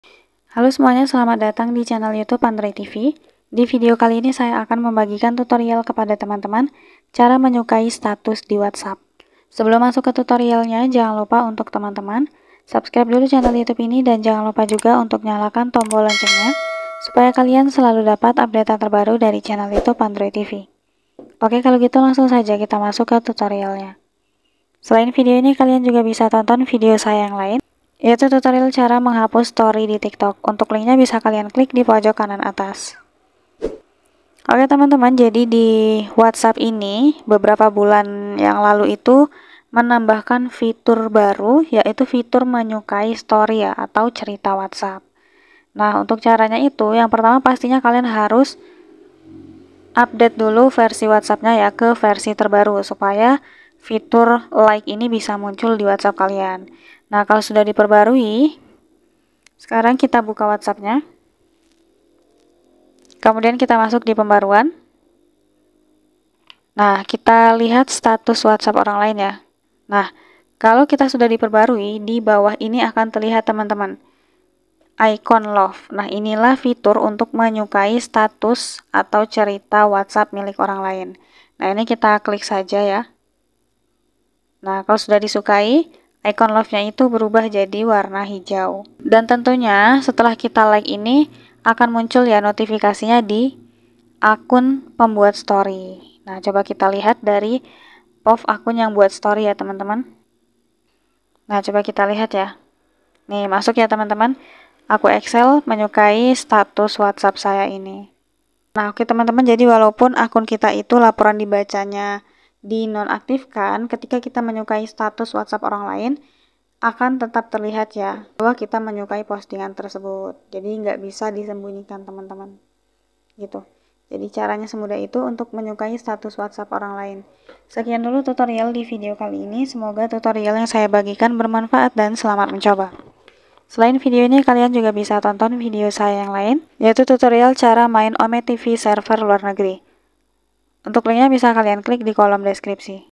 Cara Menyukai Status di WhatsApp
Cara menyukai status di WhatsApp adalah dengan menekan ikon hati yang terletak di bagian kanan bawah layar saat melihat status tersebut. Fitur ini mirip dengan Instagram Stories.